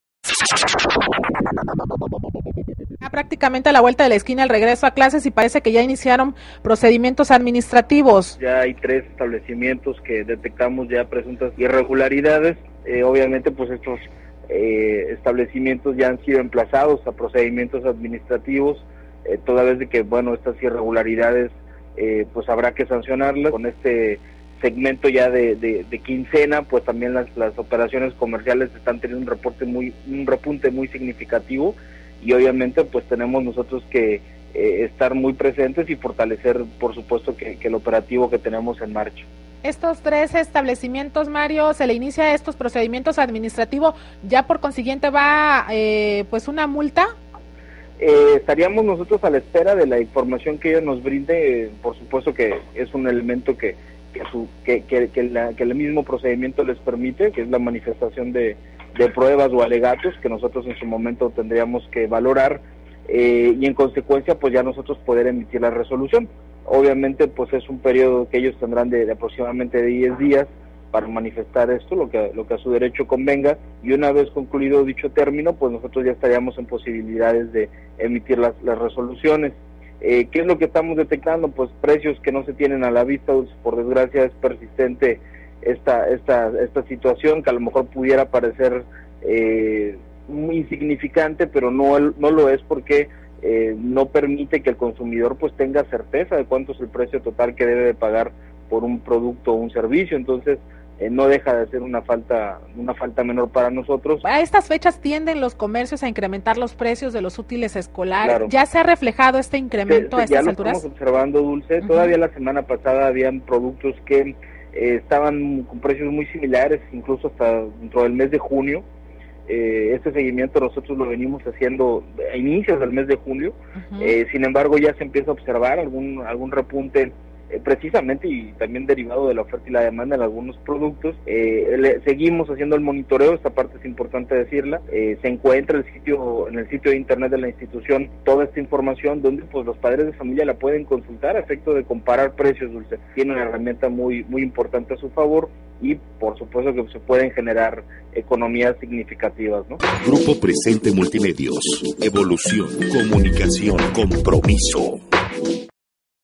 Entrevista a Mario Alva (PROFECO) por Grupo Presente Multimedios
Entrevista telefónica en el noticiero Fórmula Empresarial de Grupo Presente Multimedios a Mario Alva Ocaña Delegado de PROFECO Tabasco. Por Dulce Chávez

Operativo de regreso a clases.
16/08/2011